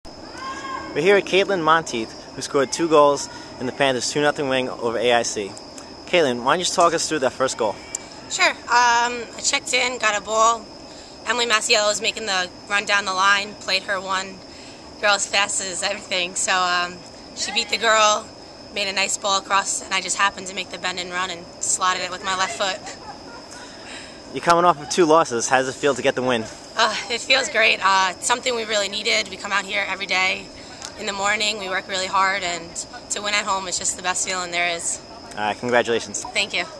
Kaitlin Monteith scores two in win over AIC
Kaitlin Monteith scored two goals in a 2-0 win over AIC to snap a two game losing streak